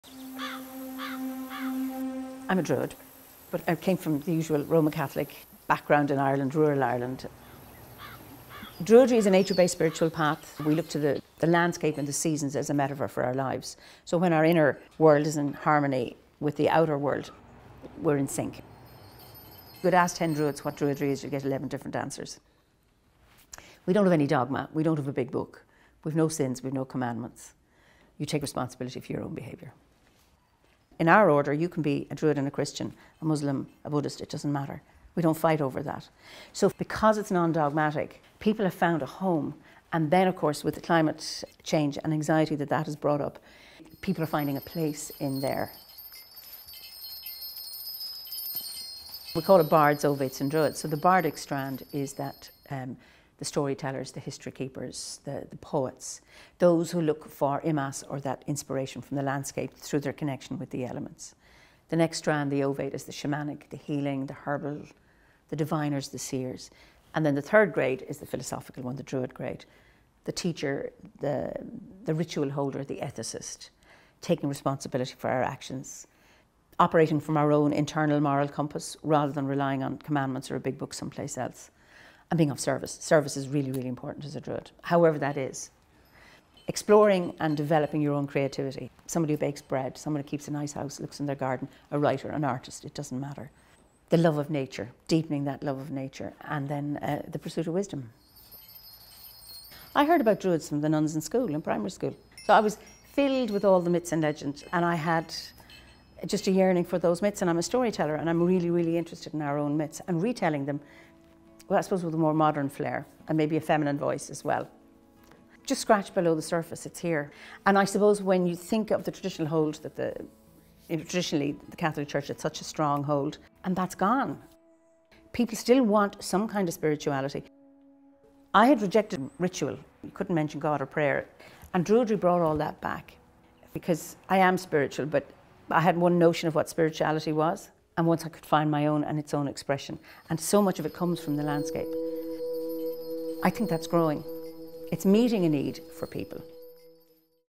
Druid Eimear Burke - ‘We’ve no dogma, no sins, no commandments’
"What I Do":  Eimear Burke is a druid based in Co. Kilkenny and she serves as chief of the Order of Bards, Ovates and Druids. Video: Bryan O'Brien